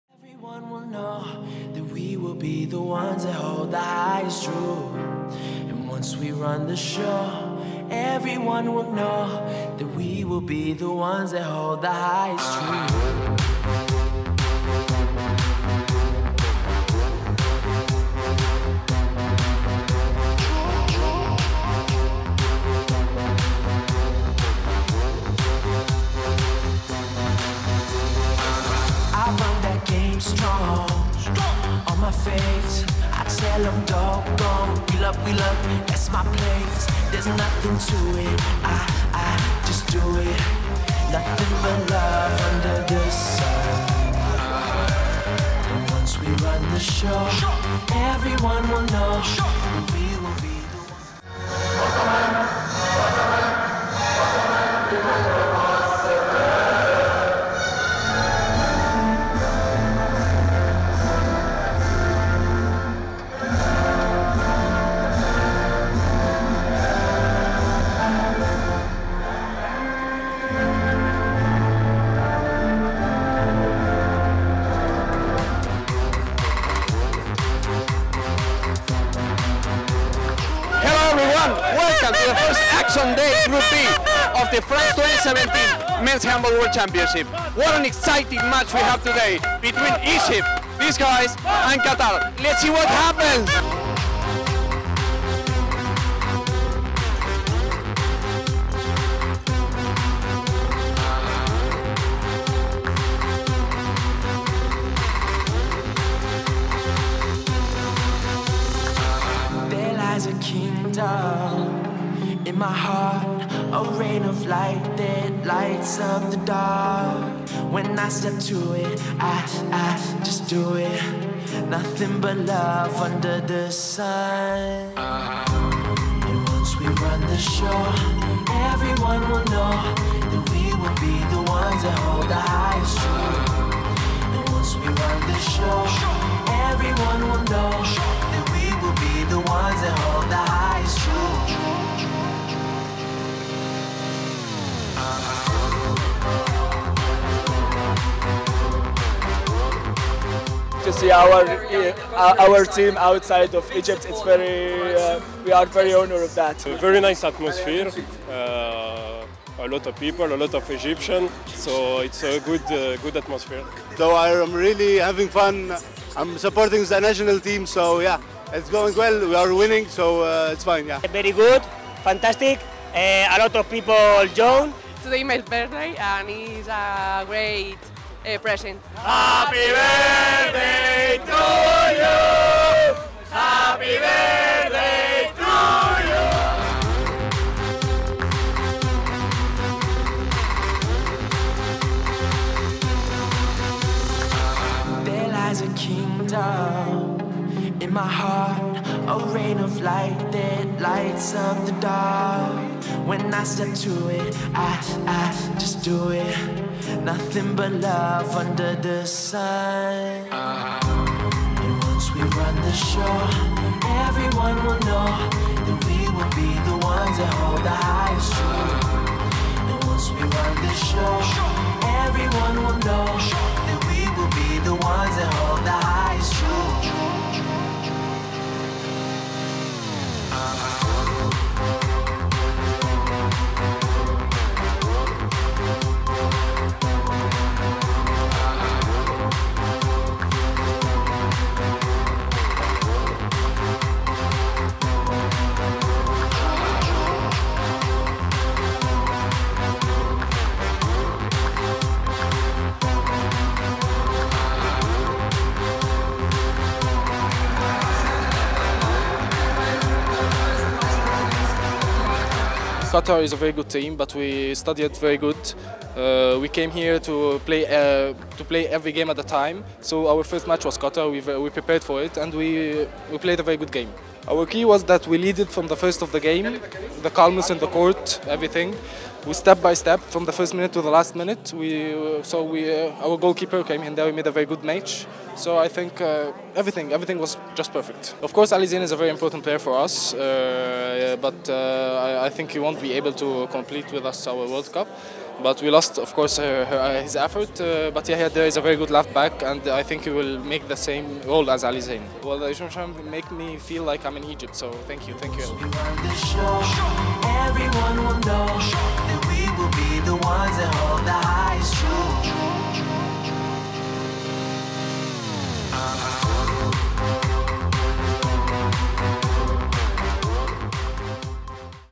Qatar 20:22 Egypt (Group D) - Highlights | IHFtv - France 2017 Men's Handball World Championship
IHFtv feature on France Qatar 20:22 Egypt. Preliminary Round, Group D. Includes an interview with Mohamed Elbassiouny (EGY).
The match that opened the 25th Men's Handball World Championship was played on January 13 in AccorHotels Arena, Paris.